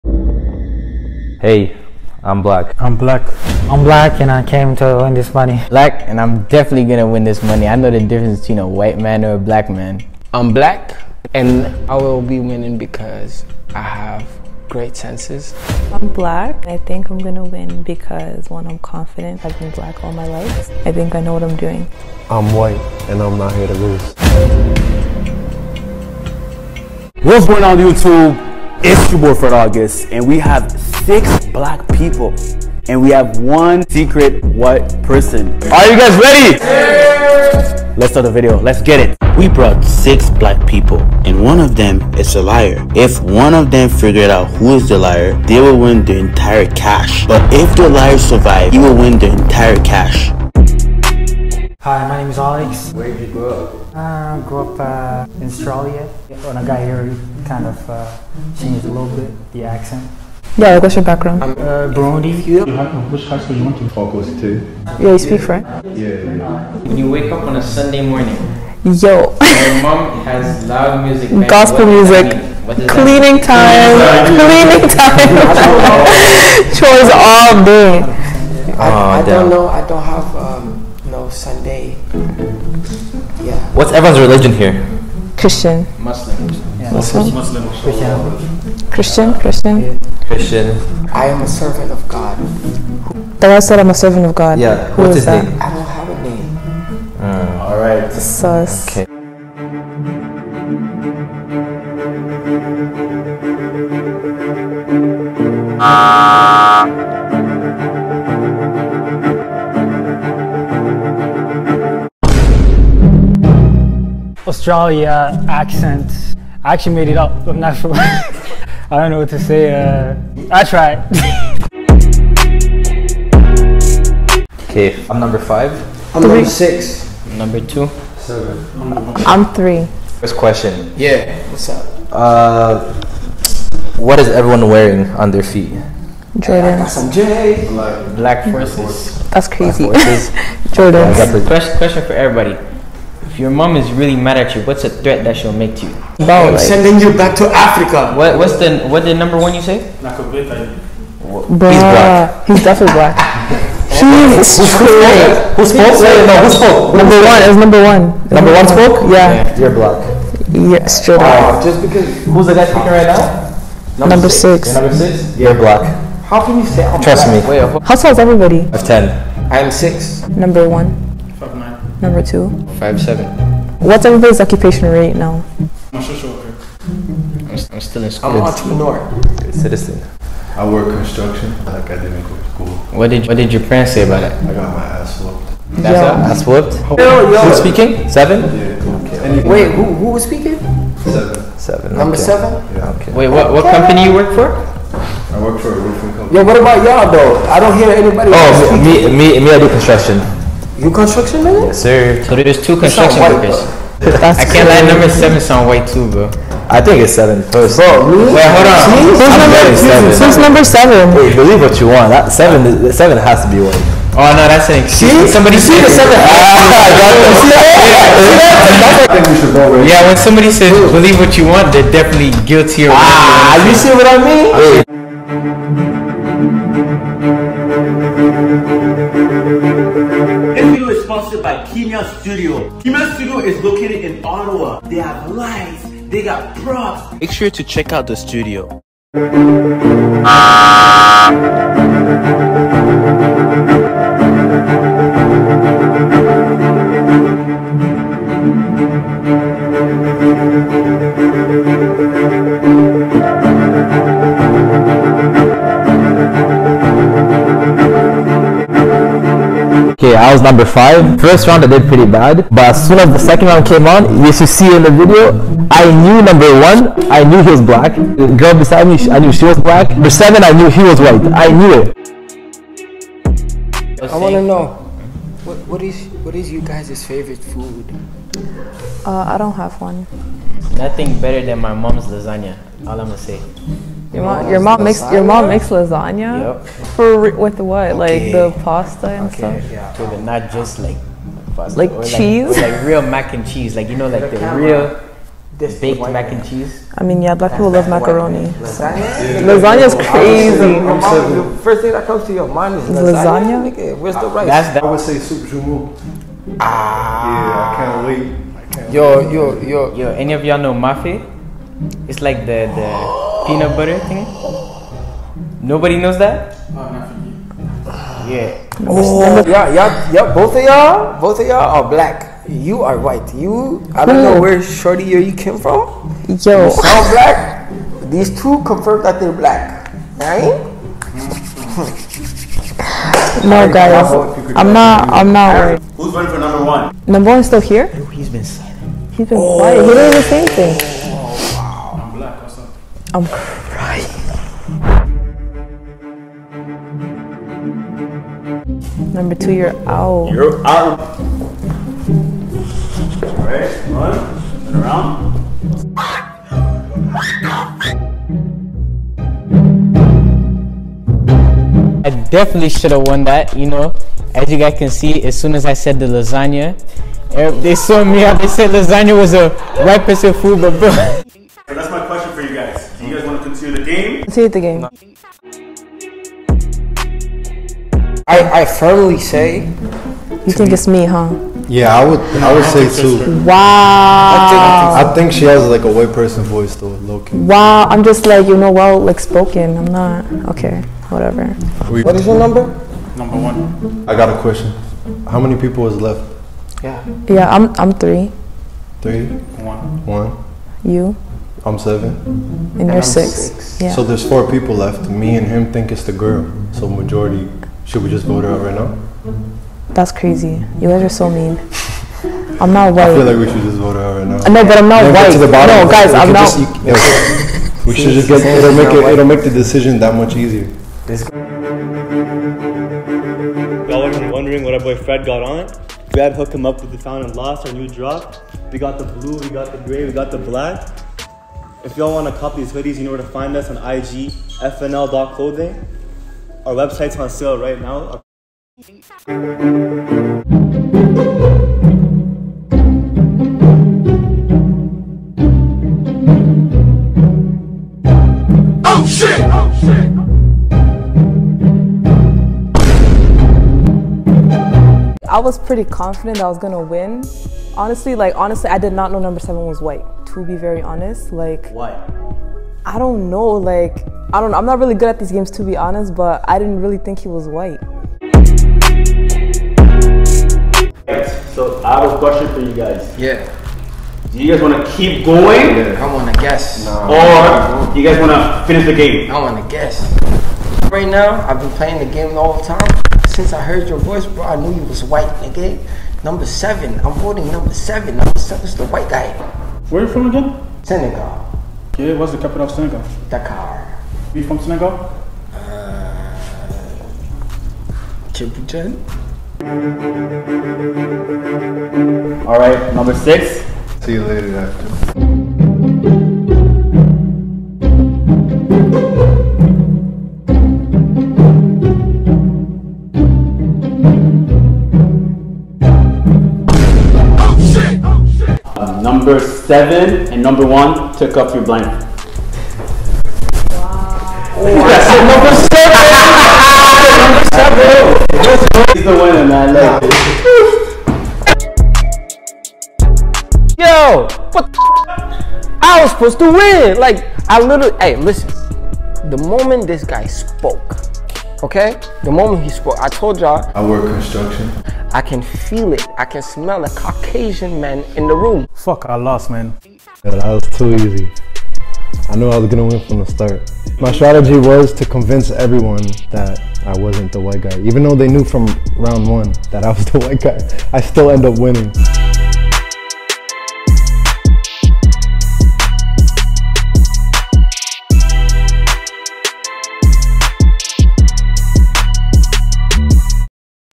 6 Black Men vs 1 Secret White Guy
6 black People vs 1 Secret White Person will participate. Everyone will be blindfolded, and the objective is to identify the White Person  individual. The person who successfully finds the White Person will win a cash prize!

6 BLACK PEOPLE VS 1 SECRET WHITE PERSON!